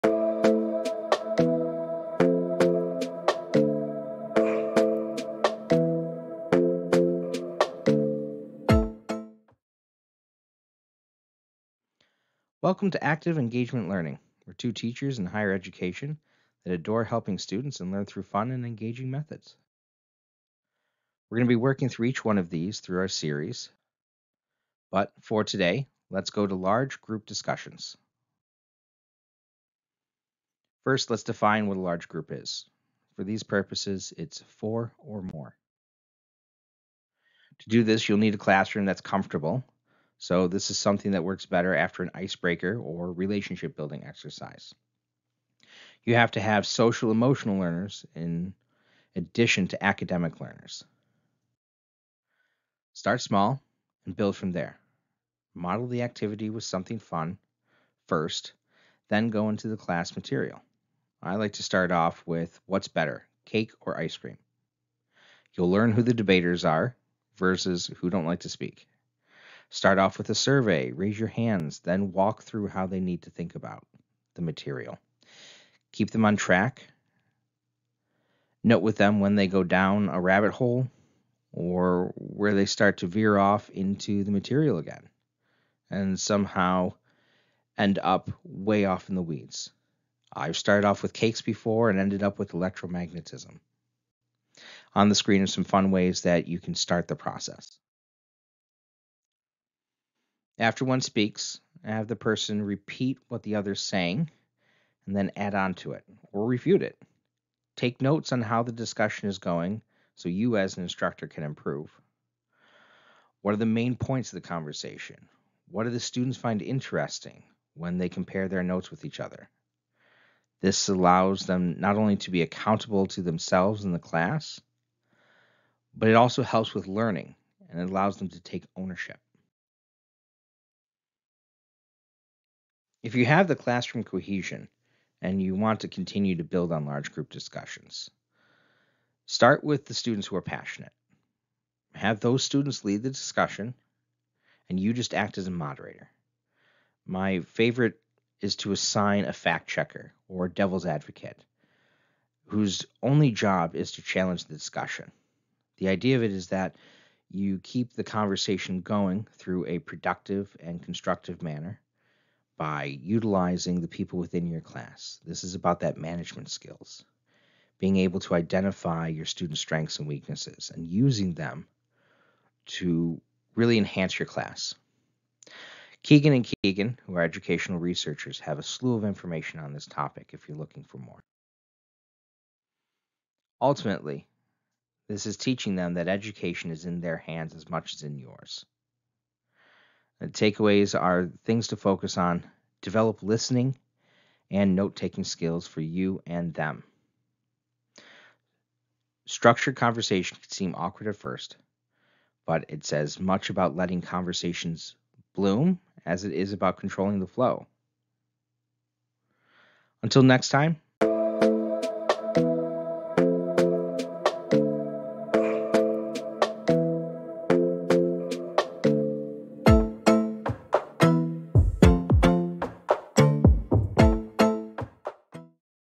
Active Learning: Teaching Large group discussions
🎓 Welcome to Active Learning, where we, as educators in higher education, are passionate about fostering engaging and interactive learning experiences for students! 📚 In today's episode, we're diving into the dynamic world of large group discussions. 💬

👥 What defines a large group, you ask? To create a conducive environment, ensure your classroom is comfortable and conducive to social and emotional engagement. 💡

🍰 But before delving into the academic material, why not kick things off with a fun icebreaker? Start with a lighthearted debate like "Cake vs. Ice Cream" to gauge your students' speaking preferences and foster participation. 🎂

🗣️ During the discussion, it's essential to keep participants on track and prevent them from veering off into unrelated topics (like going from cakes to electromagnetism!). 🧲 Utilize strategies like having students repeat and build upon each other's points to enhance engagement and comprehension. 📝

📊 Take notes on the main discussion points and observe which topics resonate most with your students. This feedback loop allows for continuous improvement in facilitating effective discussions. 📈

👩‍🏫 As you build classroom cohesion, empower passionate students to lead discussions while you take on the role of a moderator. Consider assigning roles like fact checker or devil's advocate to encourage critical thinking and constructive dialogue. 🤔

💡 Remember, education is a collaborative effort, and by leveraging your students' strengths and interests, you can create a vibrant and enriching learning environment. 🌟

🔍 For more insights into effective teaching strategies, explore resources from educational researchers like Keegan and Keegan. 📖

📝 In conclusion, focus on developing active listening and note-taking skills for both you and your students, and embrace the structured yet flexible nature of guided conversations. 🎤